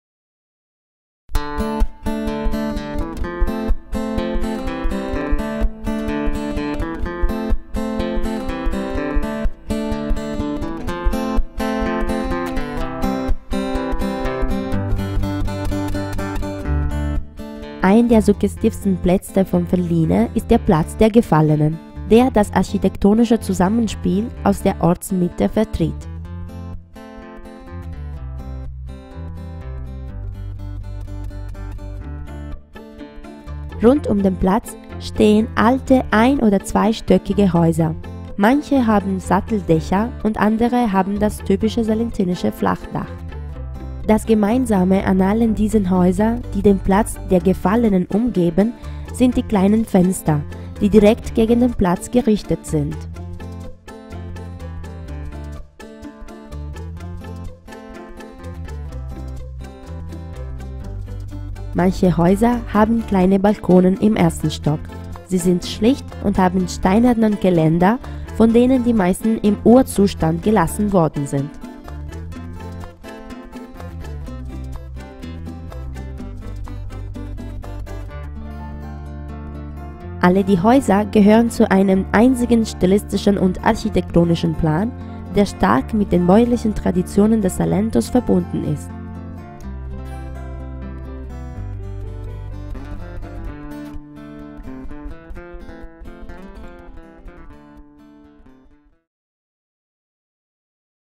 Stadt Alliste - Der Platz der Gefallenen
Ein der suggestivsten Plätze von Felline ist der Platz der Gefallenen, der das architektonische Zusammenspiel aus der Ortsmitte vertritt.
Rund um den Platz stehen alte ein- oder zweistöckige Häuser. Manche haben Satteldächer und andere haben das typische salentinische Flachdach.